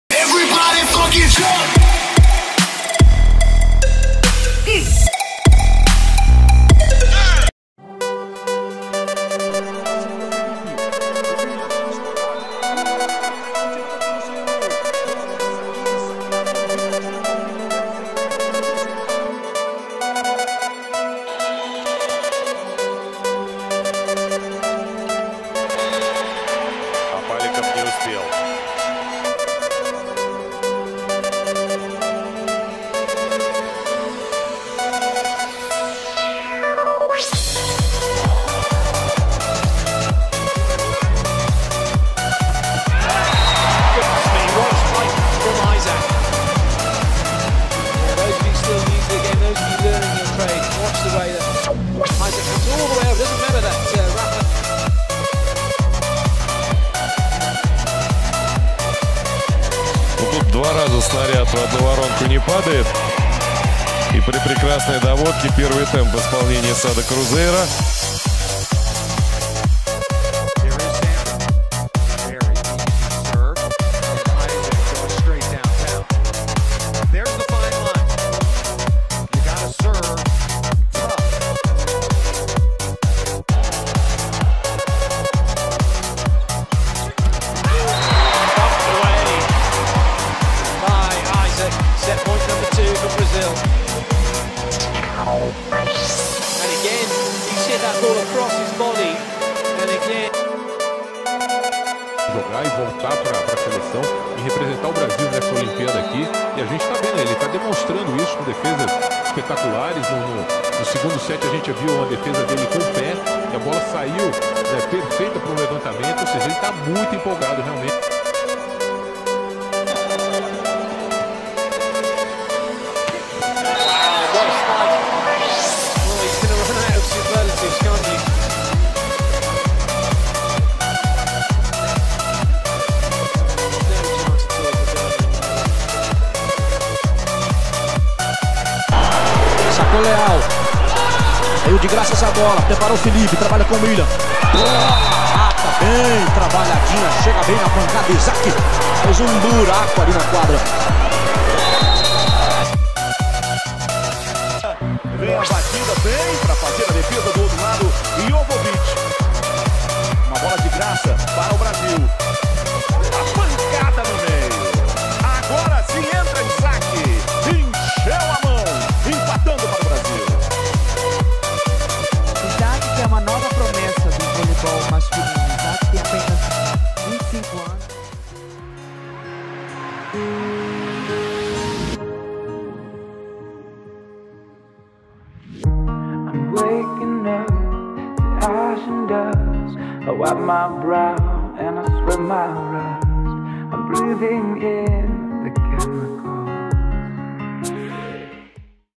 Top 10 Volleyball Attacks by Isac Santos
✔ Full name -  Isac Viana Santos
✔ Birth Date - 13/12/1990
✔ Birth Place - Rio de Janeiro, Brazil
✔ National team - Brazil
✔ Current club - Sada Cruzeiro Volei
✔ Number - 2